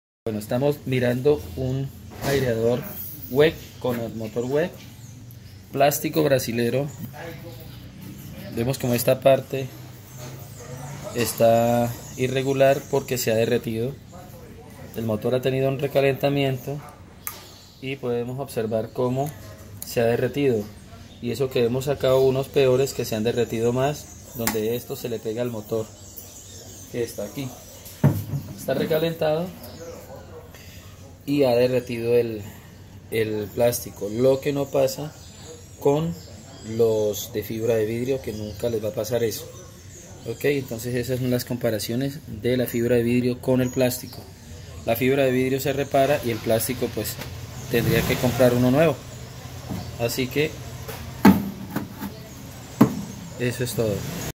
Aireador Oxipez en Fibra de Vidrio vs Aireadores de plástico otras marcas
Aireador con motor Weig Brasilero ensamblado en estructura plástica, la cual produce recalentamiento y lo derrite, lo que no pasa con los Aireador marca Oxipez en Fibra de Vidrio, los cuales son reparables y garantizados, los de ensamble plástico NO.